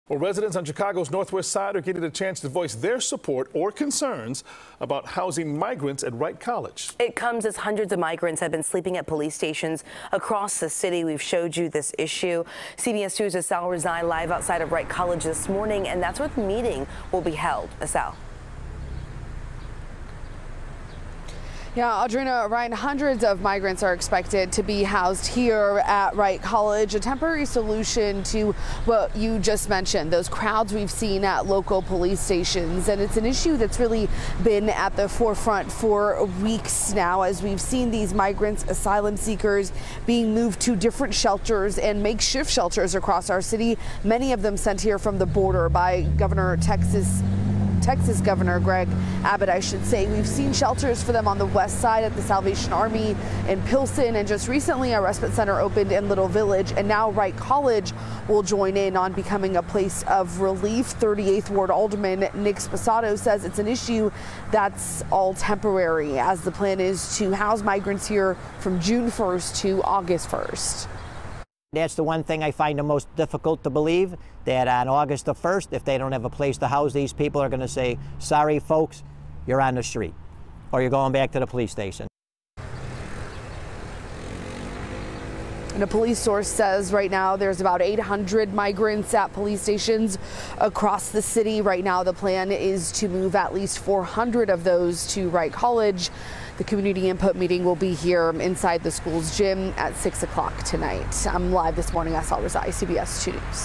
Meeting to be held Tuesday on housing migrants at Wilbur Wright College
Ald. Nick Sposato (38th) said that come June 1, hundreds of migrants will be relocated to the Northwest Side school to alleviate the crowds at local police stations.